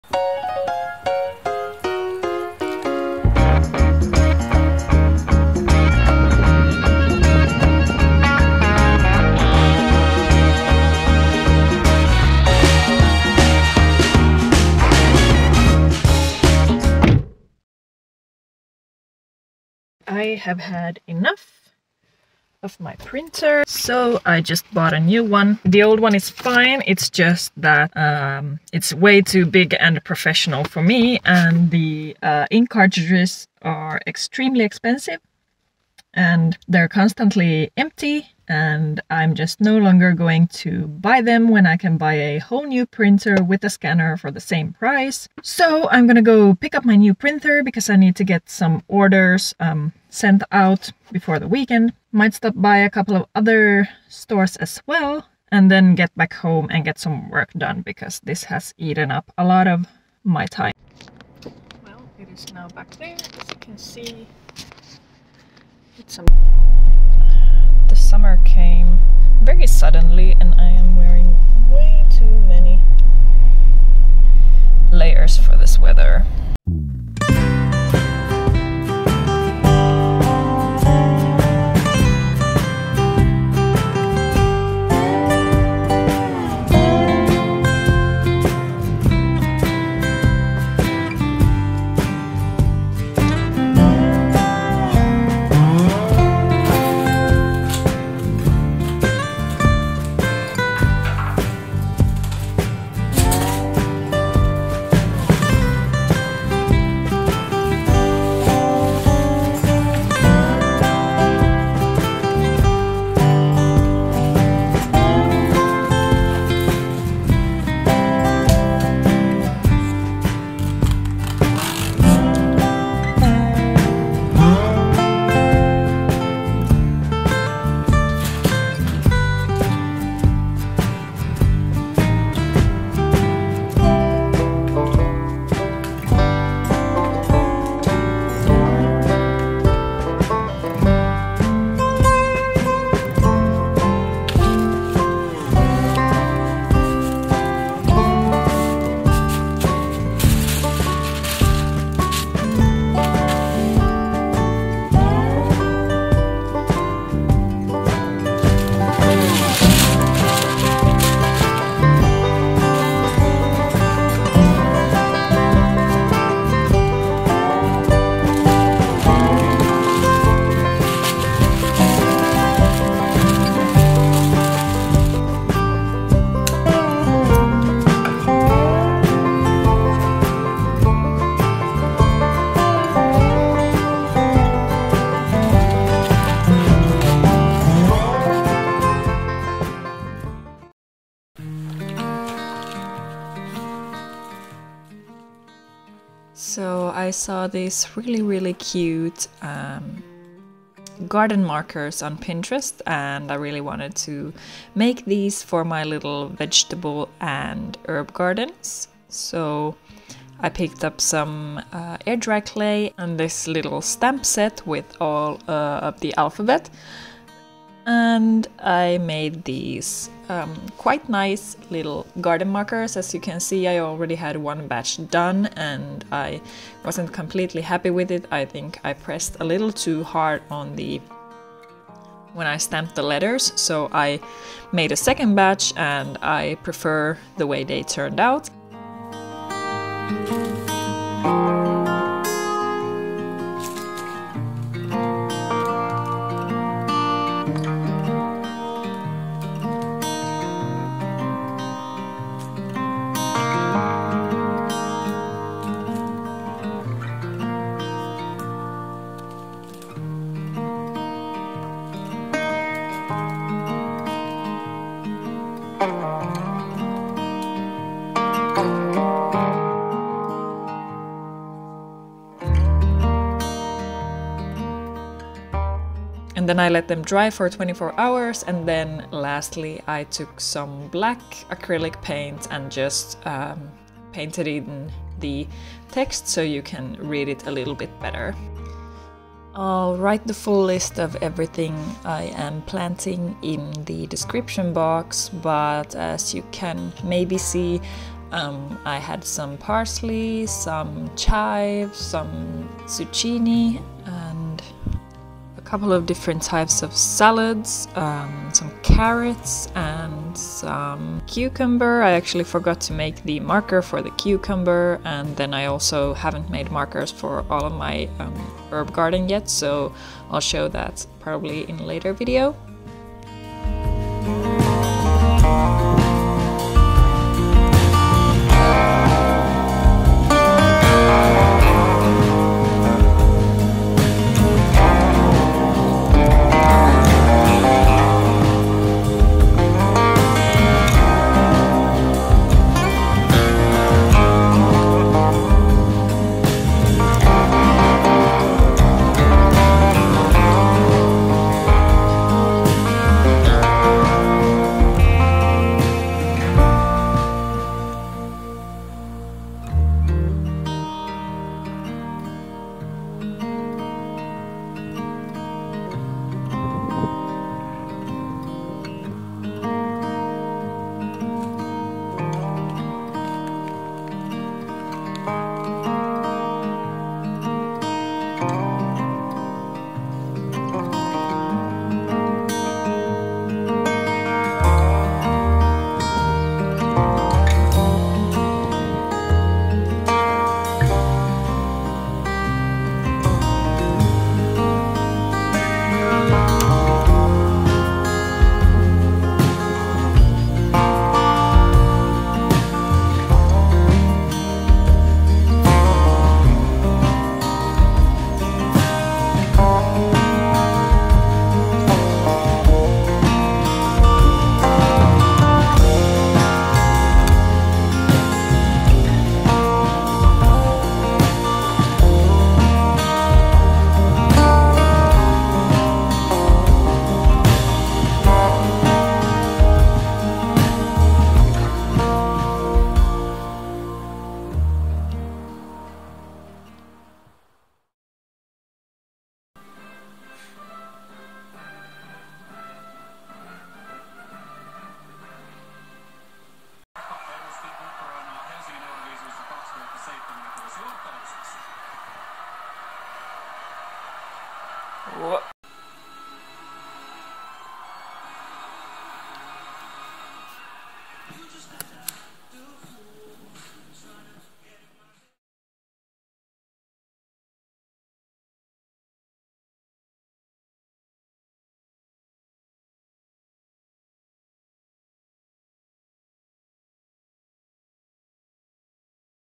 Finnish homestead vegetable garden
A little bit of art business and a whole lot of spring sowing chores in my vegetable garden.
Here I planted:
Chives
Parsley
Dill
Garden cress
Batavia lettuce
Lettuce
Cucumbers
Carrots

Could not fit the zucchinis, garlic and some other stuff here so I'll have to plant them in my second garden plot with my potatoes and pumpkins.